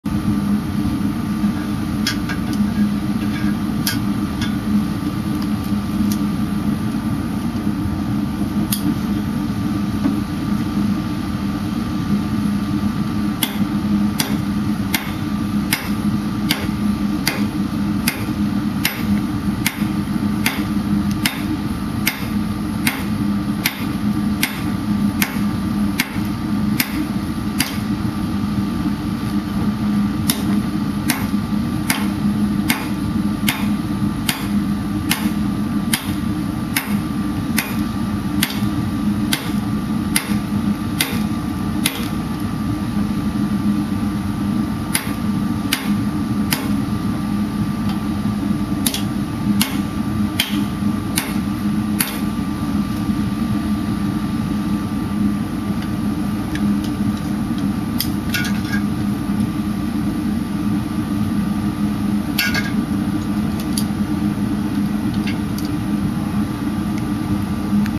gasforge2
Forging a knife out of a railroad spike with a gas forge, hammer and anvil. .